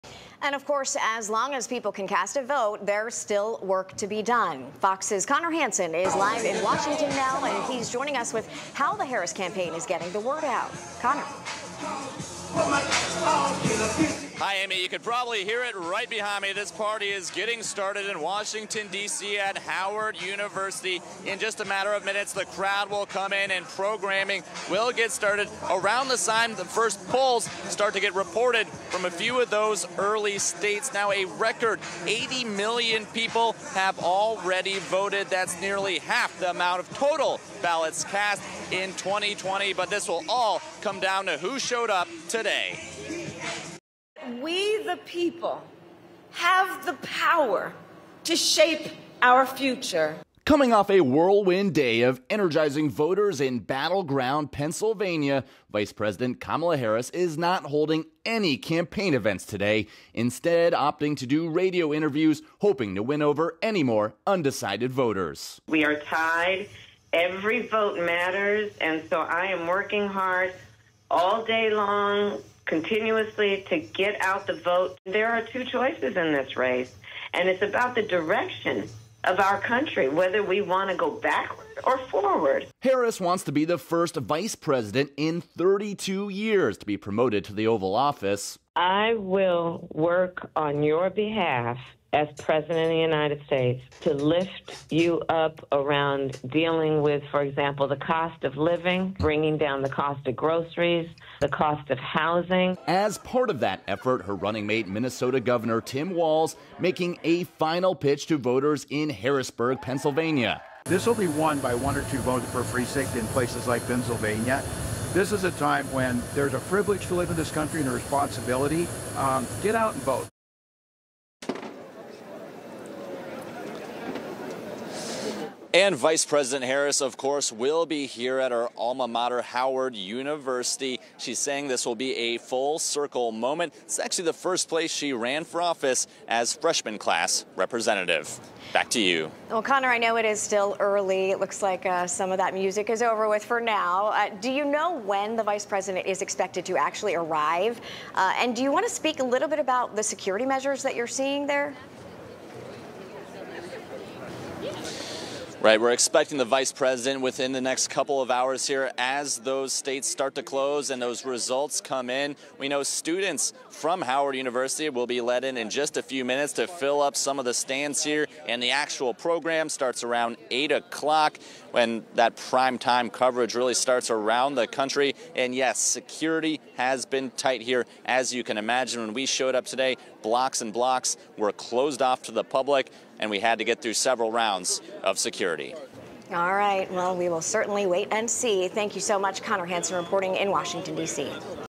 FOX News reporter Connor Hansen provides Election Day updates from Harris headquarters
Polls close in Pennsylvania at 8 p.m., which has generated the most attention by far from the Democratic and Republican presidential campaigns.